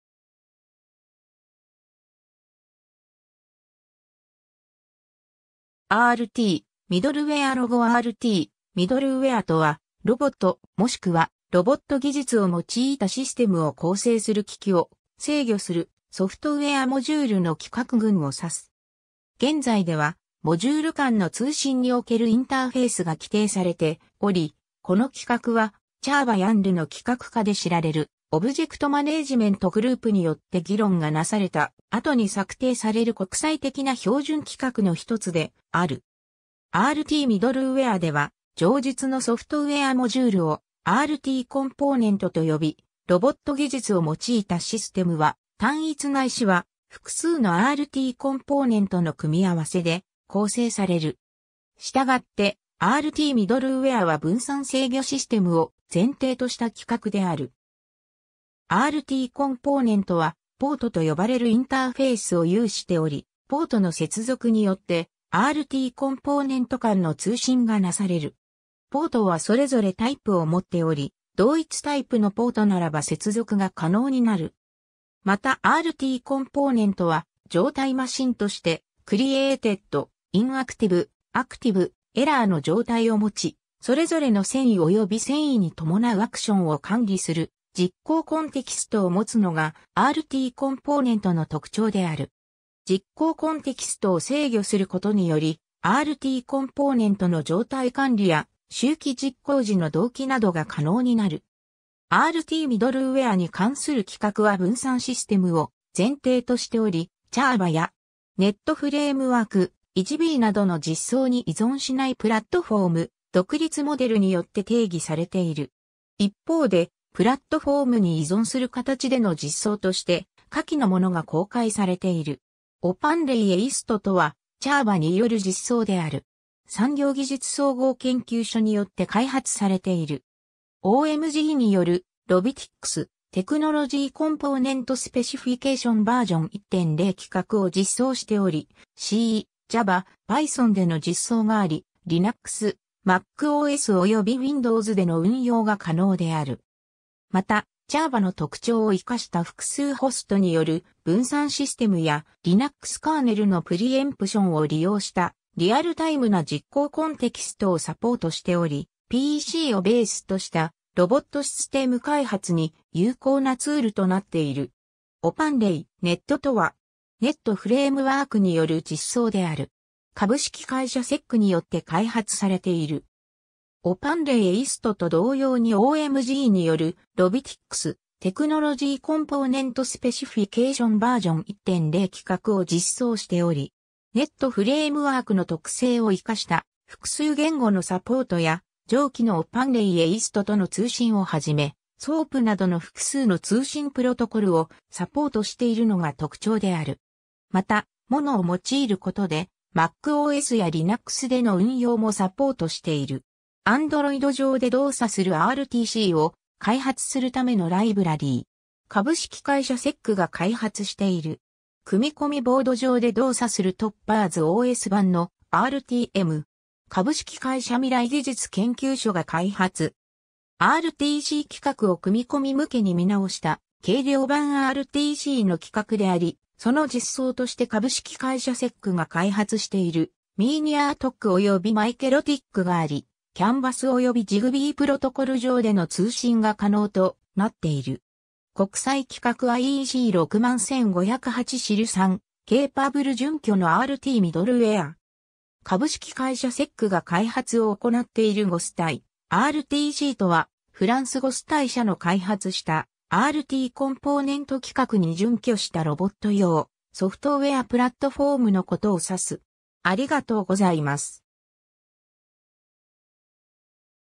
RTミドルウエア
RT-middlewareロゴ (日本ロボット工業会より引用) RTミドルウエア (RT-middleware) とは、ロボット、もしくはロボット技術 (Robotics Technologies, RT) を用いたシステムを構成する機器を制御するソフトウエア・モジュールの規格群を指す。
現在ではモジュール間の通信におけるインターフェースが規定されており、この規格は、CORBAやUMLの規格化で知られるObject Management Groupによって議論がなされた後に策定される国際的な標準規格の一つである。
 RTミドルウエアでは、上述のソフトウエア・モジュールをRTコンポーネント (RT-Component, RTC) と呼び、ロボット技術を用いたシステムは、単一ないしは複数のRTコンポーネントの組み合わせで構成される。
したがって、RTミドルウエアは分散制御システムを前提とした規格である。
 RTコンポーネントはポートと呼ばれるインターフェースを有しており、ポートの接続によってRTコンポーネント間の通信がなされる。
ポートはそれぞれタイプ（型）を持っており、同一タイプのポートならば接続が可能になる。
 またRTコンポーネントは状態マシンとして、CREATED、INACTIVE、ACTIVE、ERRORの状態を持ち、それぞれの遷移および遷移に伴うアクションを管理する実行コンテキストを持つのがRTコンポーネントの特徴である。
実行コンテキストを制御することにより、RTコンポーネントの状態管理や、周期実行時の同期などが可能になる。
 RTミドルウエアに関する規格は分散システムを前提としており、CORBAや.NET Framework、EJBなどの実装に依存しないプラットフォーム独立モデル (PIM) によって定義されている。
一方で、プラットフォームに依存する形での実装として、下記のものが公開されている。
 OpenRTM-aistとは、CORBAによる実装である。
産業技術総合研究所によって開発されている。
OMGによるRobitics Technology Component Specification version 1.0規格を実装しており、C++、Java、Pythonでの実装があり、Linux, Mac OSおよびWindowsでの運用が可能である。
また、CORBAの特徴を生かした複数ホストによる分散システムや、Linuxカーネルのプリエンプションを利用したリアルタイムな実行コンテキストをサポートしており、PCをベースとしたロボットシステム開発に有効なツールとなっている。
 OpenRTM.NETとは、.NET Frameworkによる実装である。
株式会社セックによって開発されている。
OpenRTM-aistと同様にOMGによるRobitics Technology Component Specification version 1.0規格を実装しており、.NET Frameworkの特性を生かした複数言語のサポートや、上記のOpenRTM-aistとの通信をはじめ、SOAPなどの複数の通信プロトコルをサポートしているのが特徴である。
また、Monoを用いることでMac OSやLinuxでの運用もサポートしている。
 Android上で動作するRTCを開発するためのライブラリ。
株式会社セックが開発している。
 組込みボード上で動作するTOPPERS OS版のRTM。
株式会社未来技術研究所が開発。
 RTC規格を組み込み向けに見直した軽量版RTCの規格であり、その実装として株式会社セックが開発しているminiRTCおよびmicroRTCがあり、CANバスおよびZigBeeプロトコル上での通信が可能となっている。
 国際規格IEC 61508 SIL3 Capable準拠のRTミドルウエア。
株式会社セックが開発を行っている Gostai RTCとは、フランスGostai社の開発したRTコンポーネント規格に準拠したロボット用ソフトウエアプラットフォームのことをさす。